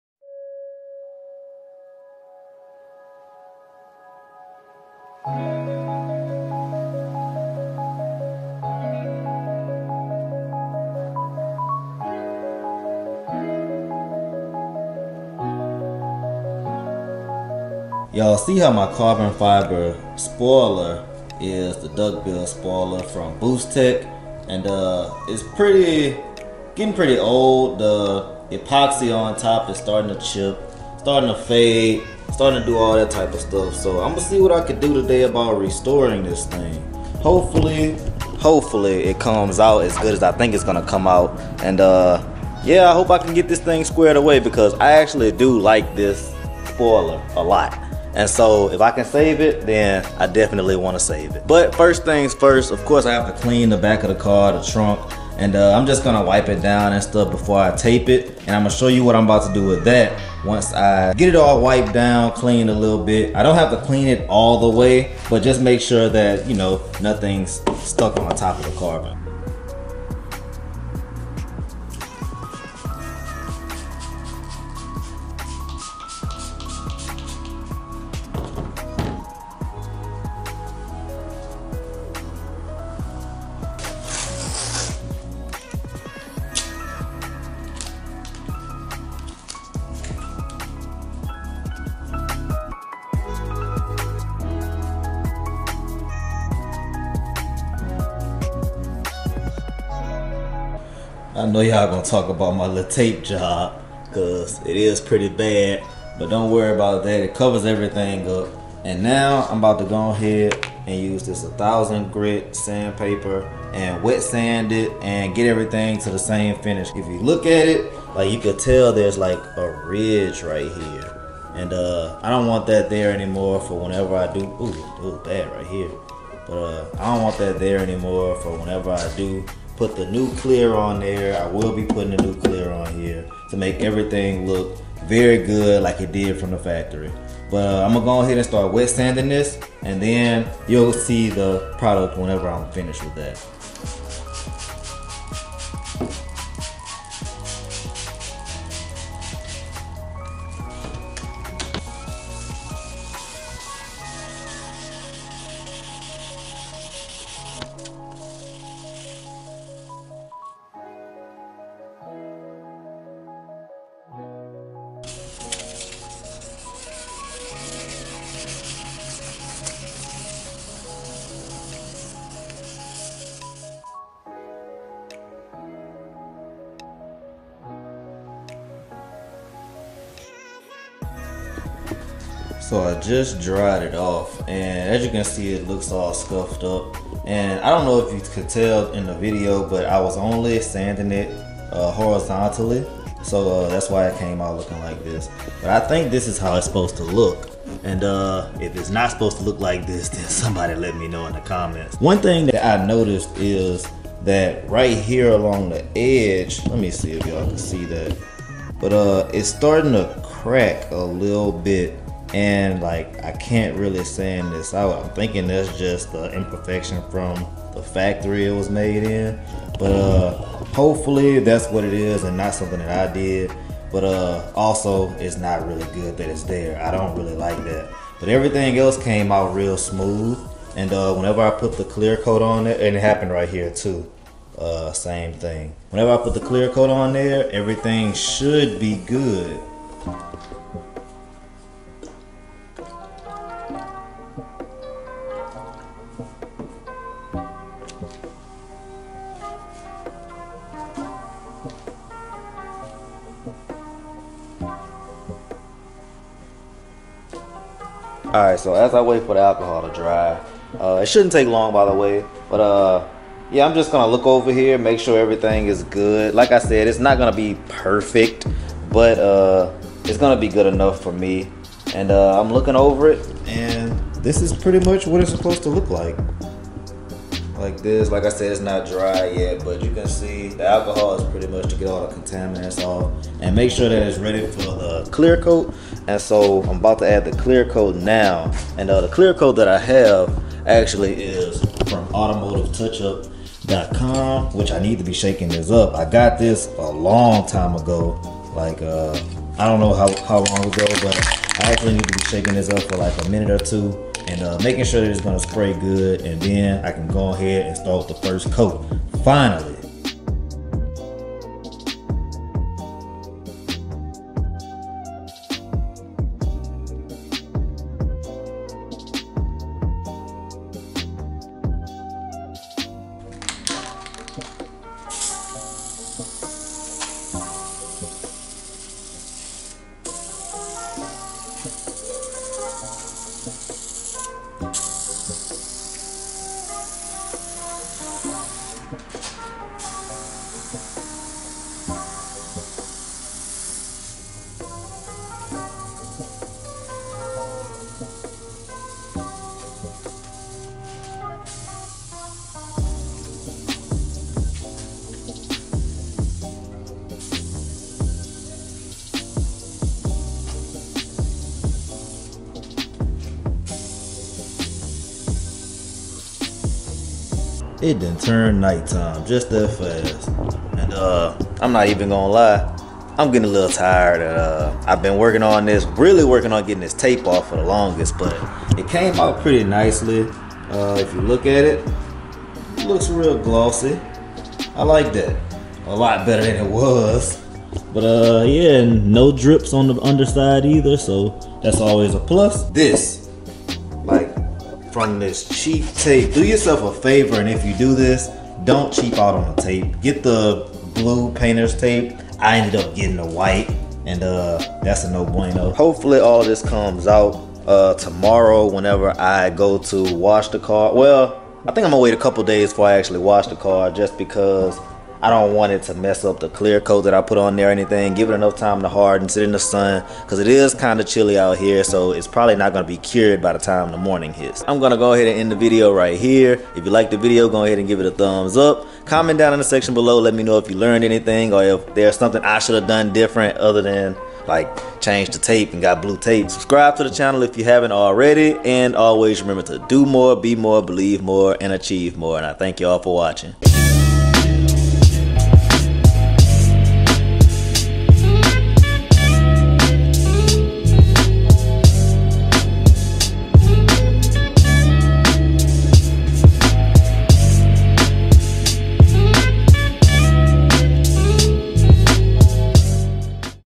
How to RESTORE Faded Carbon Fiber | Making Old Carbon Fiber Look NEW
This is a short video showing my faded carbon fiber duckbill spoiler and a diy of how I fixed this. The process of restoring clear coat isn't one to be afraid of, and it's super simple if you wanted to tackle it yourself. Side note, just make sure you take your time wet sanding and it'll come out fine.